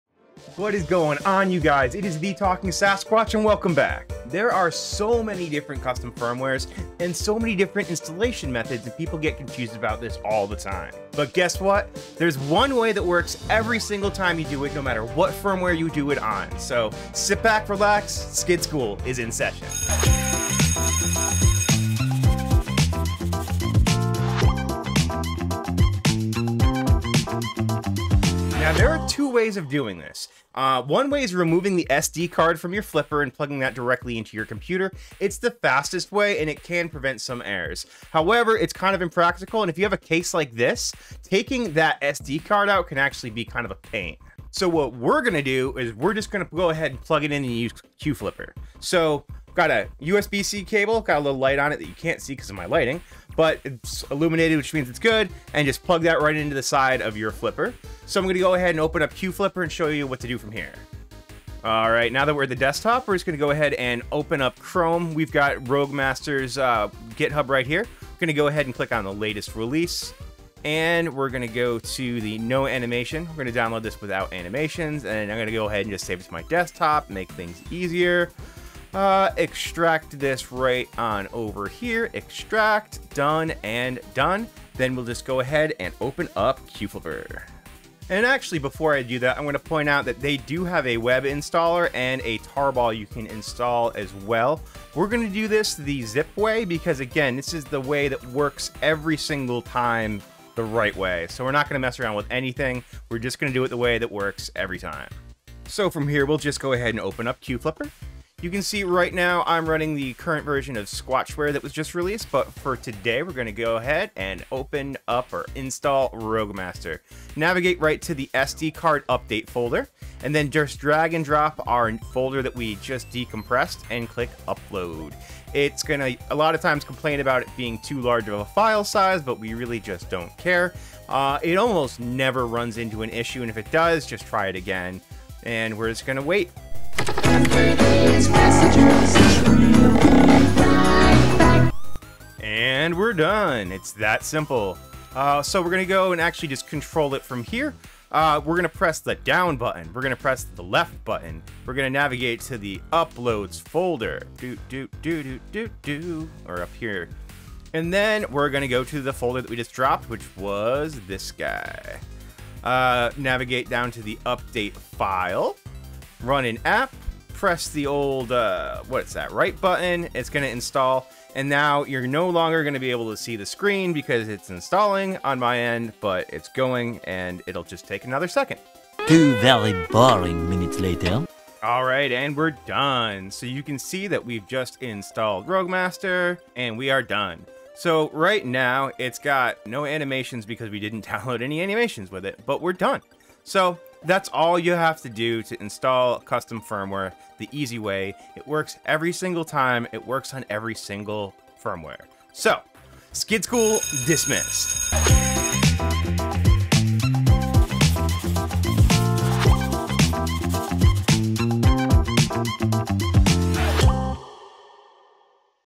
Skid School 102 : Install Any Custom Firmware on Flipper Zero!! Works 100% of the Time! 🐬🐱‍💻➡ 🐱‍🏍😸
This week's Skid School is going to cover the BEST method to install ANY Custom Firmware on the Flipper Zero simply by downloading the release from GitHub and installing it using the update folder!! Couldn't be easier!

Amazon List of All the Parts I Use